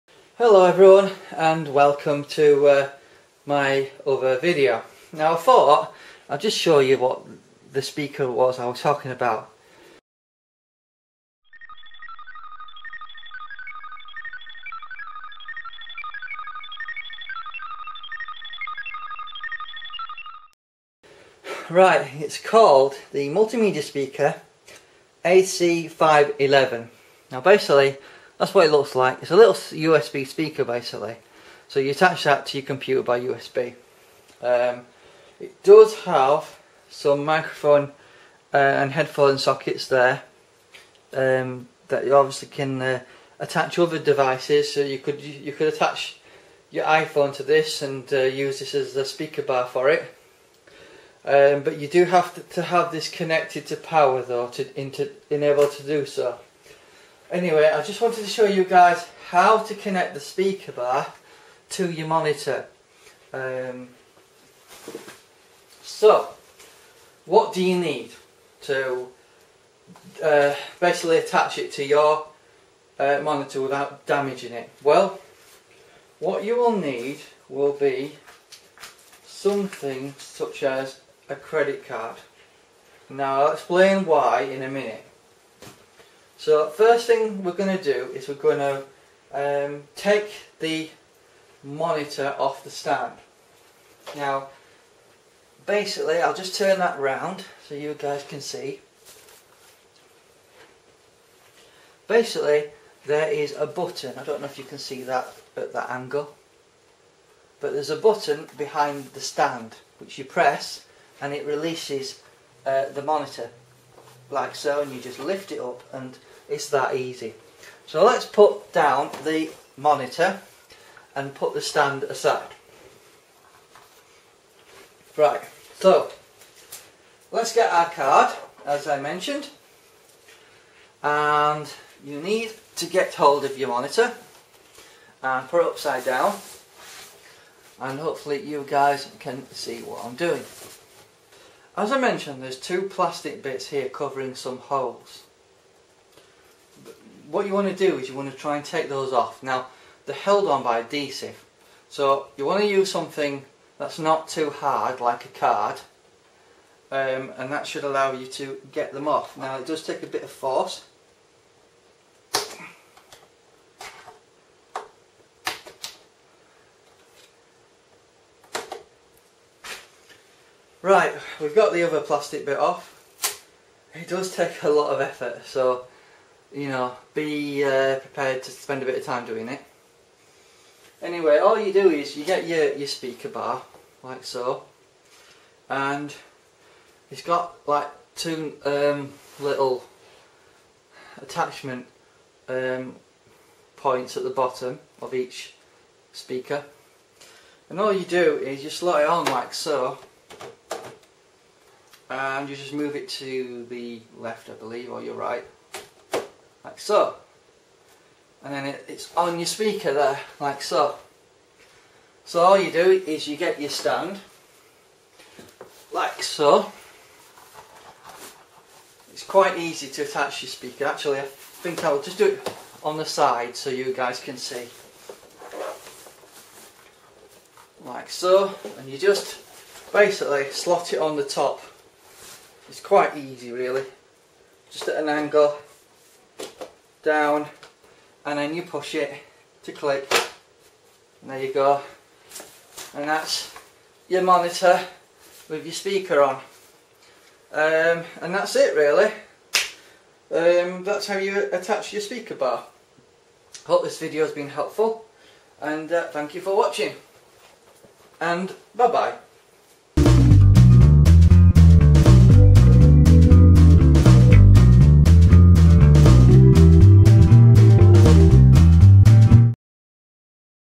Dell ac511 speaker How to install
Hear I show you how to install the Dell AC511 speakers that go on the monitor 2714H and possibly other models as well. To purchase speaker in UK